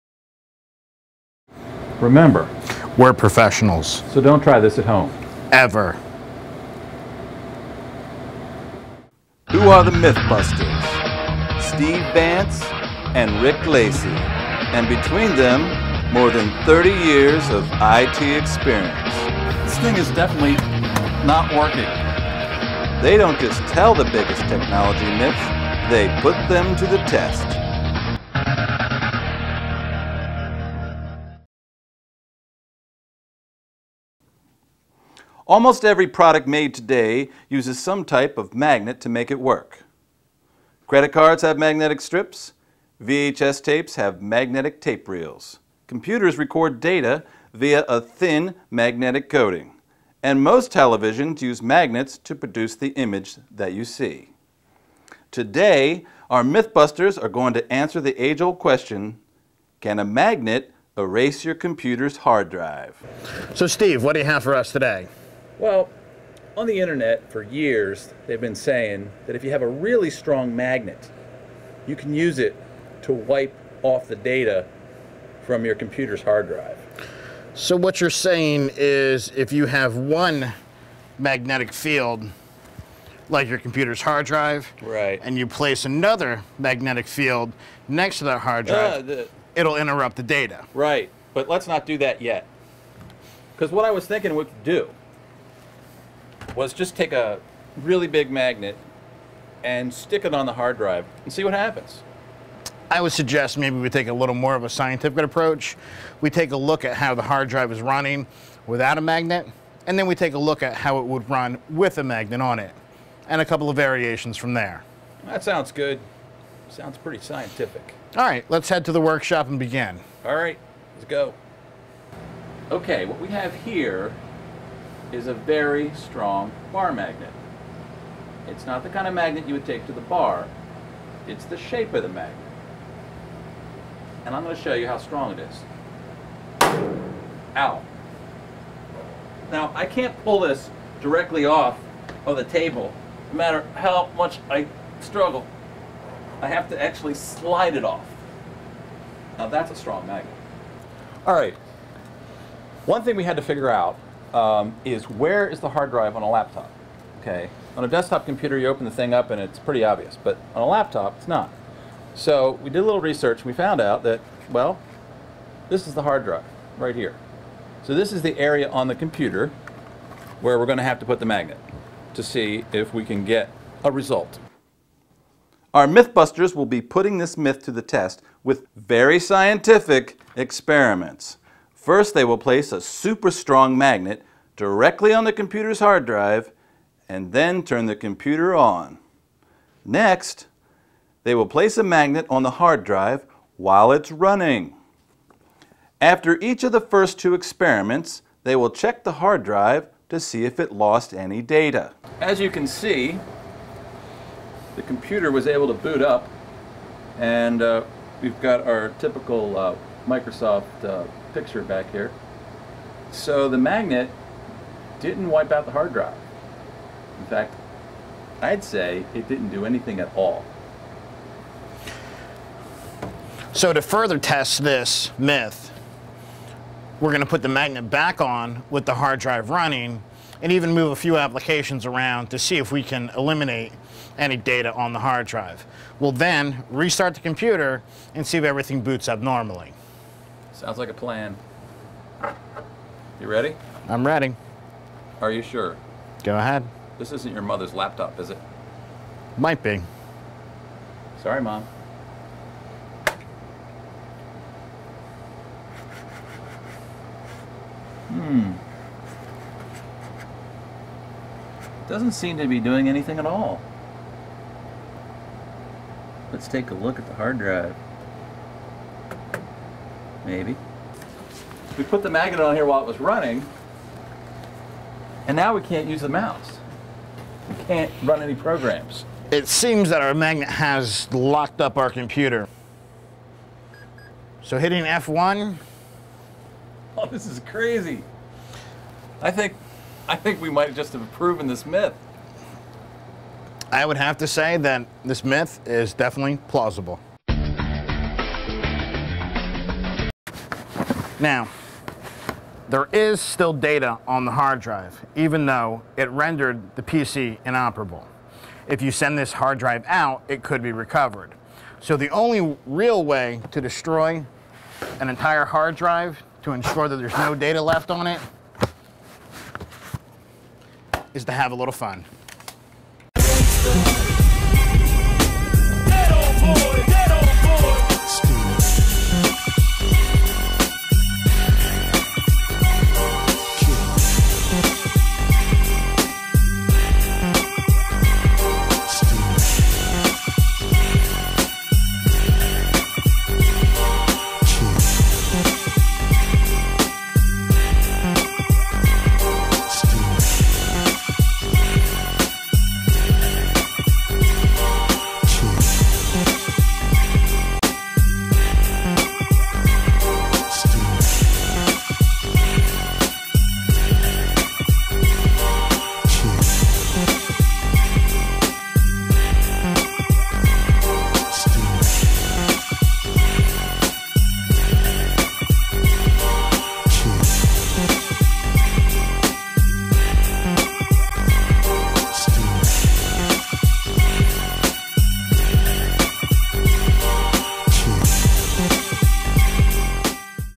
MythBusters Parody - Can a Magnet ruin your laptop's hard drive?
Can a magnet ruin your computer's harddrive?